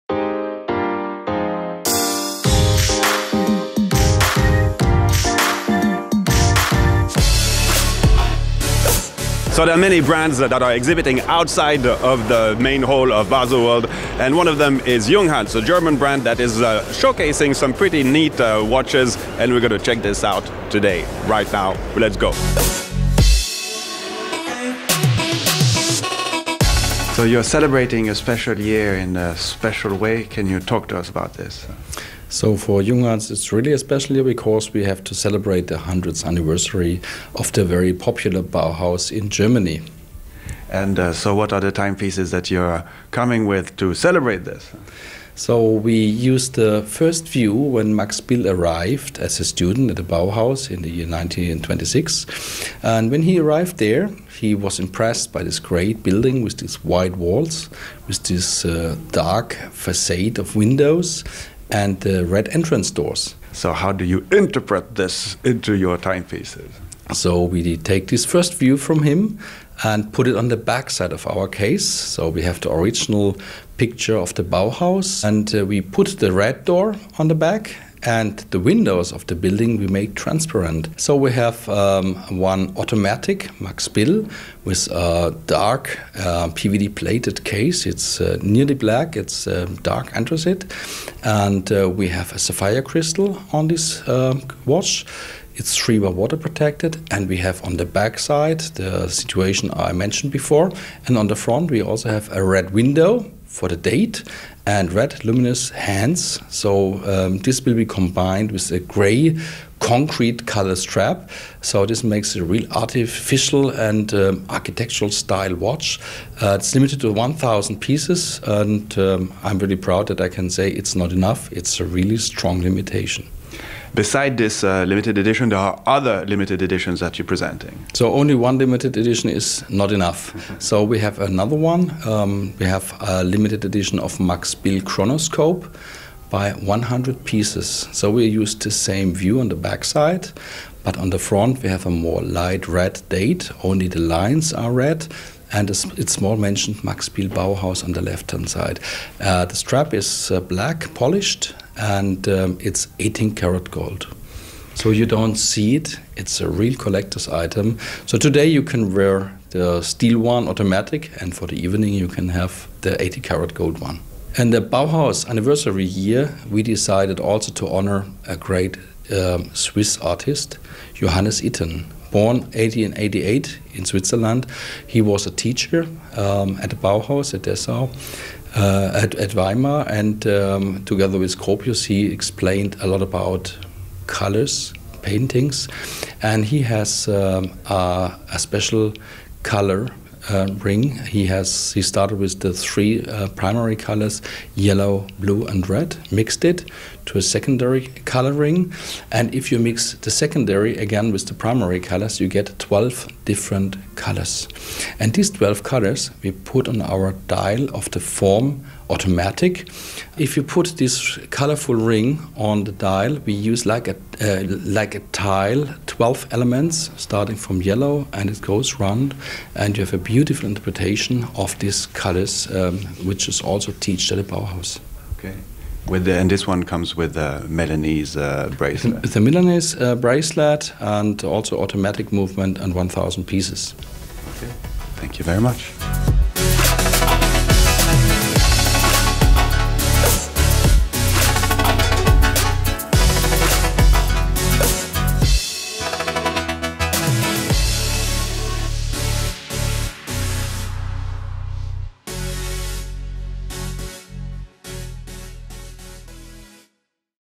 Junghans Pays Tribute to Bauhaus at Baselworld 2019
To commemorate the 100th anniversary of the Bauhaus movement, German brand Junghans introduced 3 commemorative watches in the Max Bill collection. To mark this, they have for instance used sapphire glasses instead of the regular plexiglas, but also a special white gold edition and a multi coloured dial design one.

I personally really like this brand and there is much to be said when it comes to decently priced watches with an attitude regarding Junghans: simple, efficient and pretty timeless.